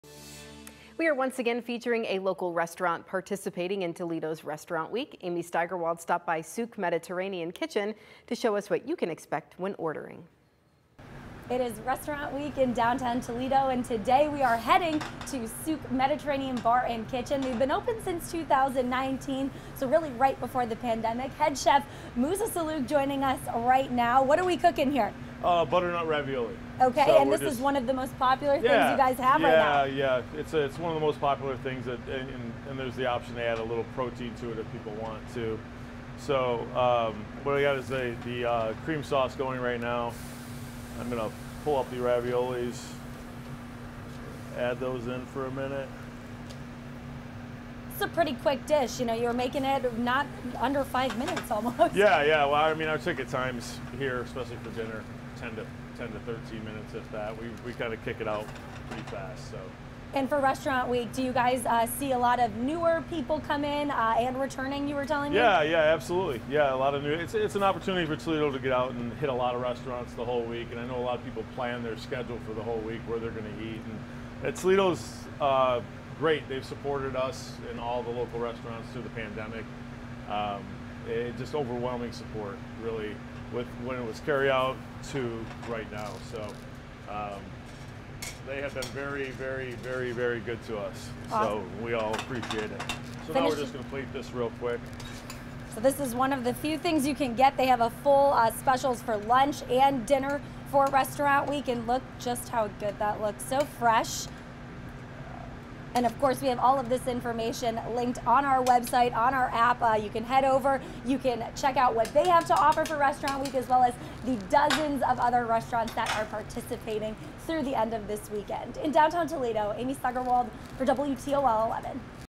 Restaurant Week 2022 Spotlight: Souk Mediterranean Kitchen
Souk serves a variety of different options, including a Saturday brunch.

They encourage reservations, especially on the weekend. Upon reopening after the shutdown they were a very popular spot downtown and continue to attract a crowd on a weekly basis! For restaurant week they have a $15 lunch special and a $30 dinner special which goes through Saturday

Here's more on what Souk is cooking up during Restaurant Week.